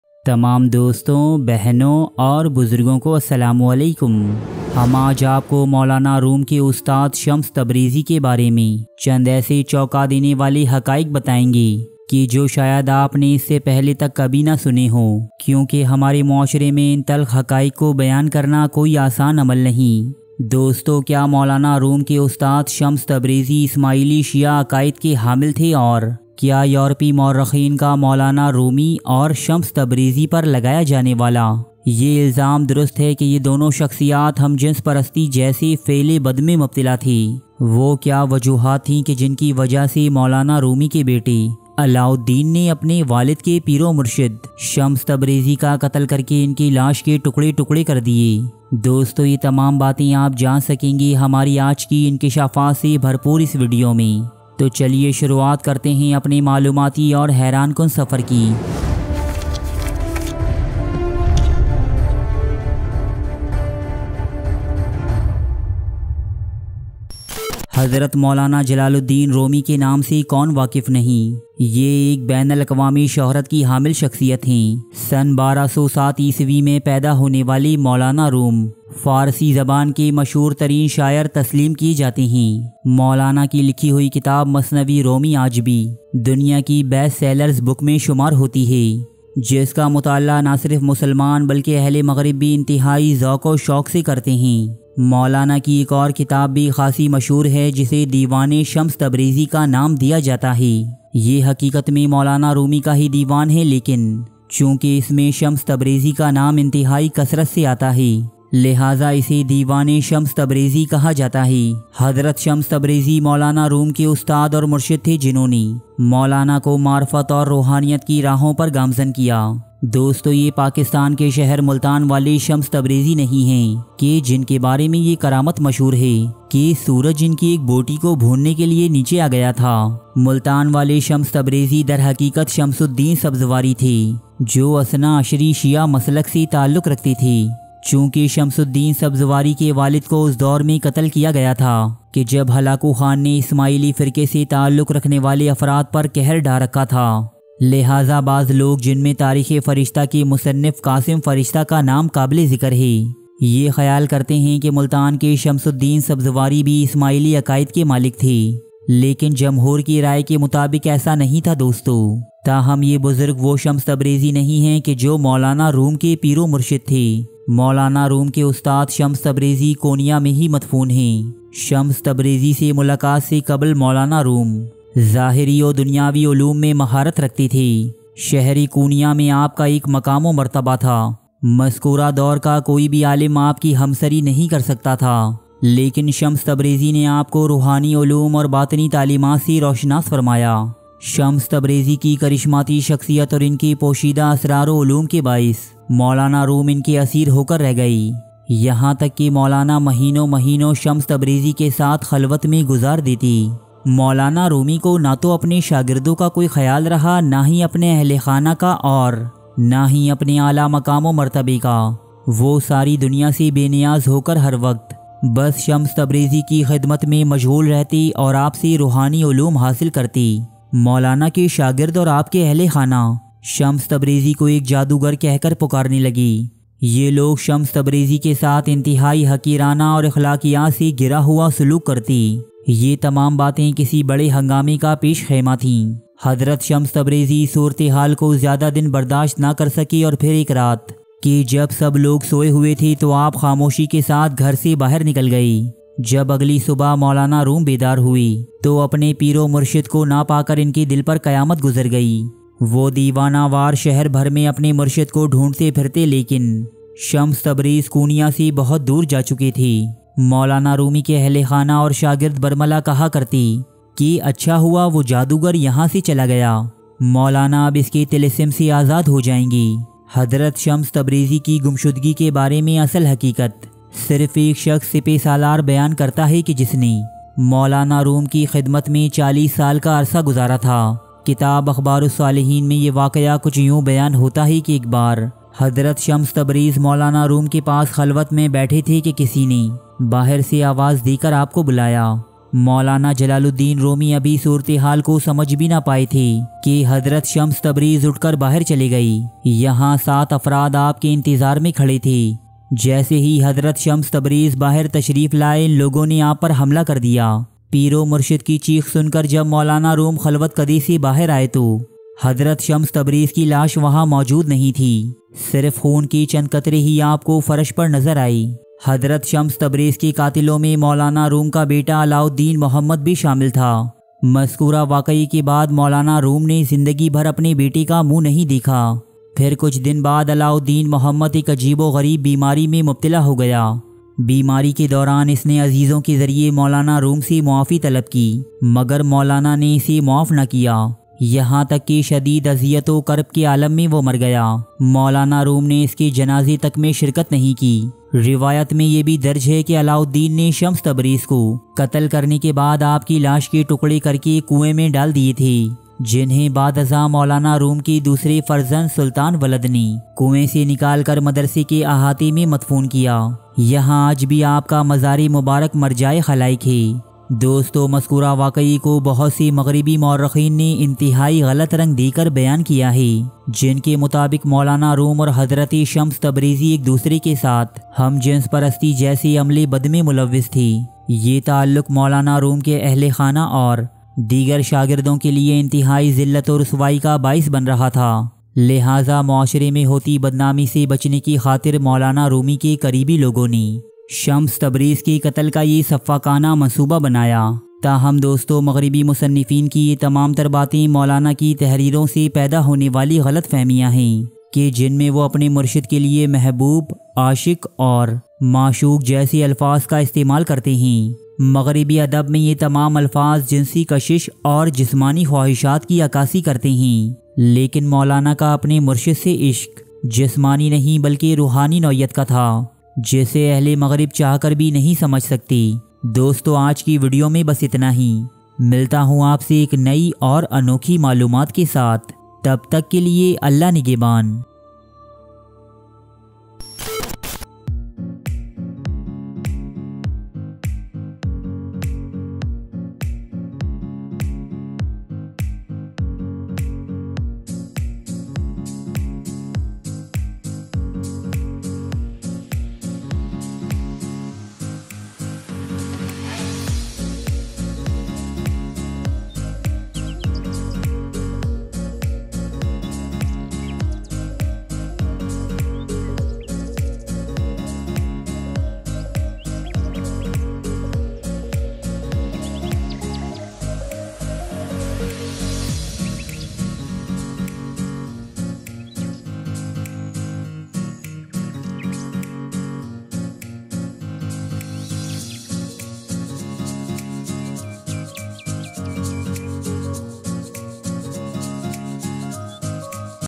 Story of Moulana Jalal Uddin Rumi and Shams Tabrizi in Hindi & Urdu
Asslam o Alekum Dosto...! Humari aaj ki Video Tarikh e islam ke 2 ehem tareen kirdaron aur in ke taluq par mabni hai, Dosto Molana Jalal uddin rumi ke murshid w peer Shams tabrizi kon the, kiya ye ek ismaili shia the? in ka qatal khud molana ke bete Alauddin ne kiyon kiya tha, kiya angrezon ko maulana par lagaya jane wala hum jins parasti ka ilzam durust hai agar nahin tu phir is ilzam ki bunyad kiya hai..? dosto ye sab aur aise bohut se inkeshafat aap jaan saken ge humari aaj ki is Heran kun Video main. mazeed malomat ke liye Mukammal Video mulahiza kijiye...